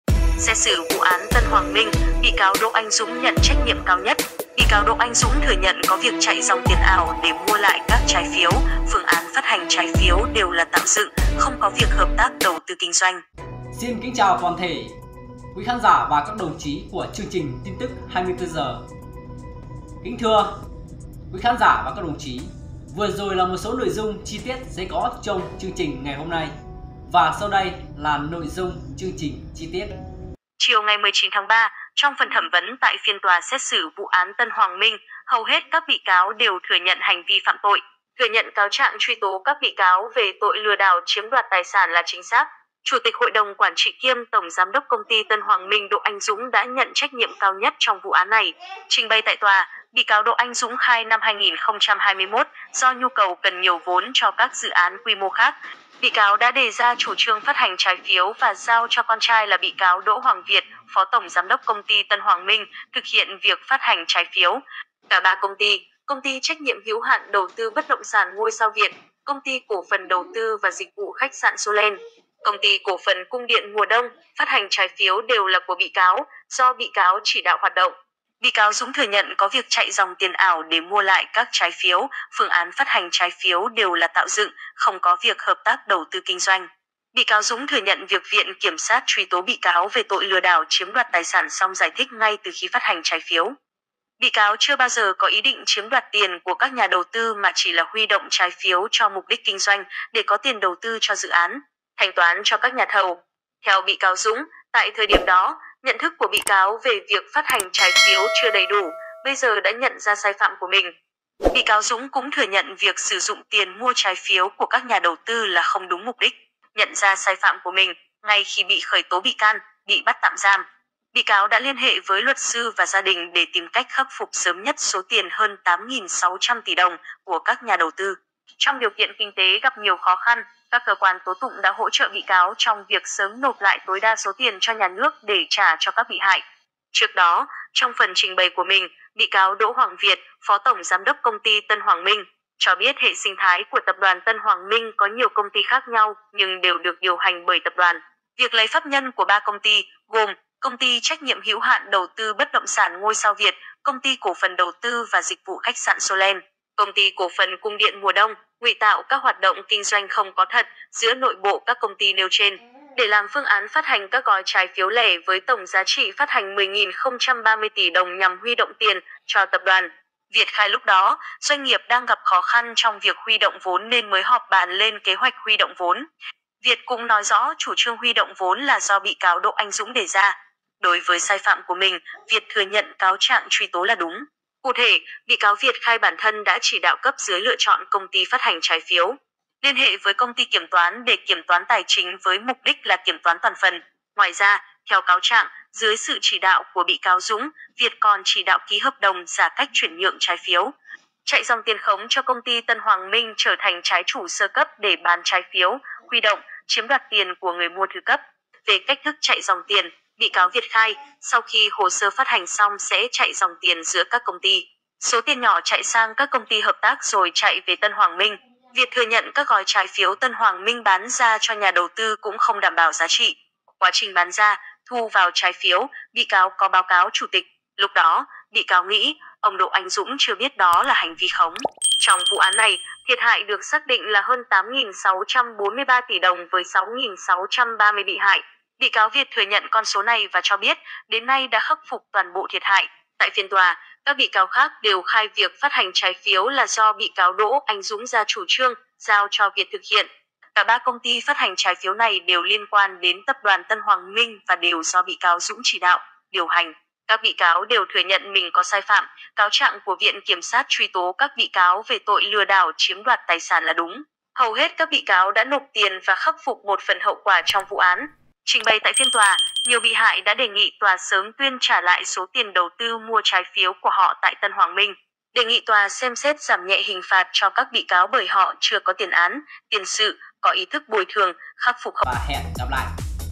Xét xử vụ án Tân Hoàng Minh: Bị cáo Đỗ Anh Dũng nhận trách nhiệm cao nhất
Chủ tịch Hội đồng quản trị kiêm Tổng Giám đốc Công ty Tân Hoàng Minh Đỗ Anh Dũng đã nhận trách nhiệm cao nhất trong vụ án này.

Trình bày tại tòa, bị cáo Đỗ Anh Dũng khai năm 2021, do nhu cầu cần nhiều vốn cho các dự án quy mô khác, bị cáo đã đề ra chủ trương phát hành trái phiếu và giao cho con trai là bị cáo Đỗ Hoàng Việt (Phó Tổng Giám đốc Công ty Tân Hoàng Minh) thực hiện việc phát hành trái phiếu.